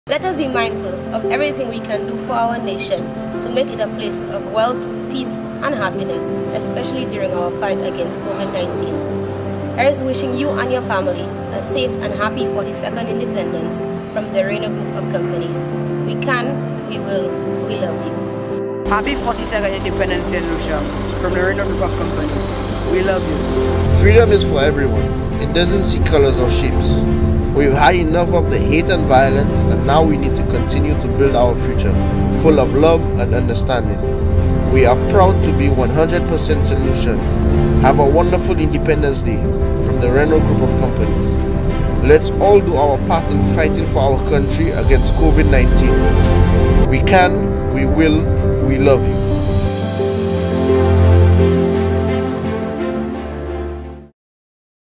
Happy 42nd Anniversary of Independence Saint Lucia from Rayneau Group of Companie
Happy 42nd Anniversary of Independence Saint Lucia from Rayneau Group of Companies...
Featured here Savvy Gajadhar (General Manager-Rayneau Rentals & Sales), Paresh Ray Gajadhar (Apprentice), Mokesh Ray Gajadhar (Business Development Executive - RG Quarry)